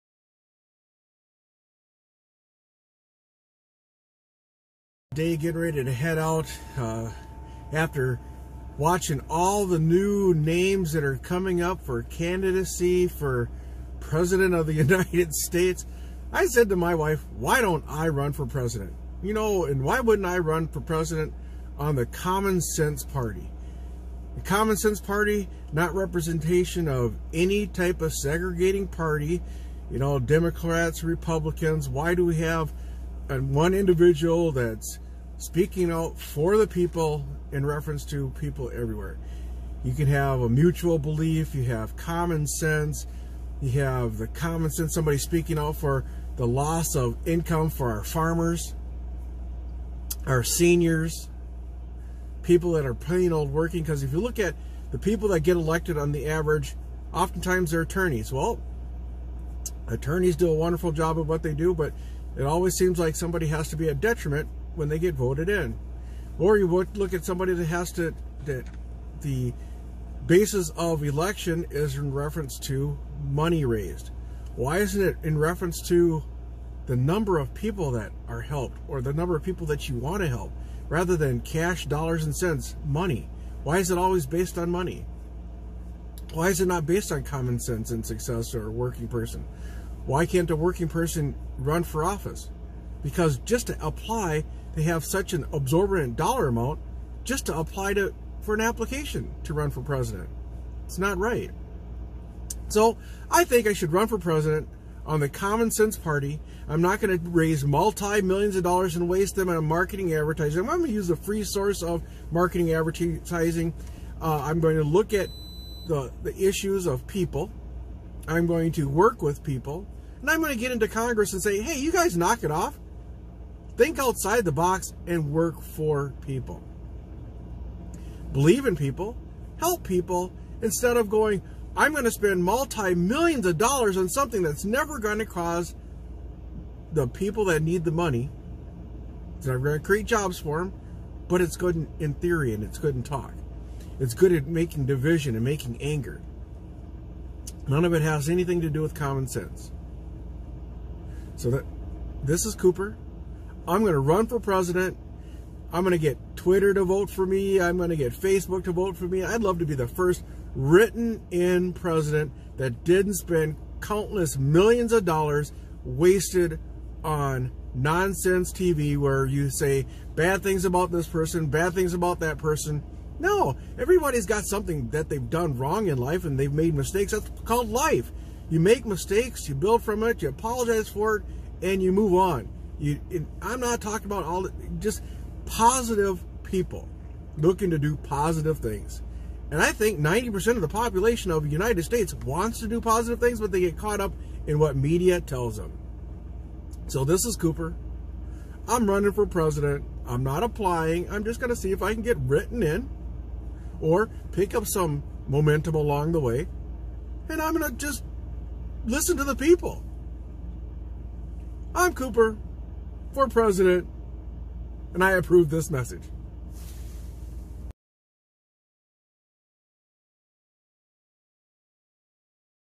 Cooper for President
Cooper for President. What if I were able to run on the common sense party? Voting would be based on helping people and not how much money you have or can raise!